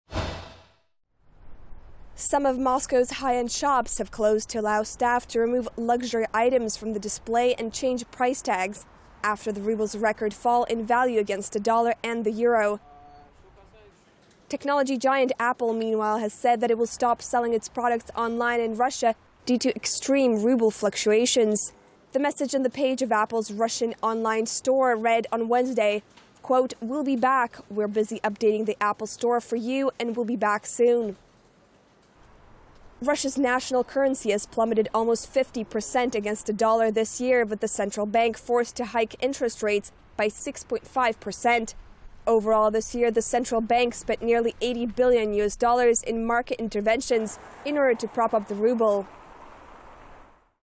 Russian Ruble Crisis: Luxury Moscow boutiques raise prices over Kremlin currency collapse
Some of Moscow's high-end shops have closed to allow staff to remove luxury items from the display and change price tags after the ruble’s record fall in value against the dollar and the euro.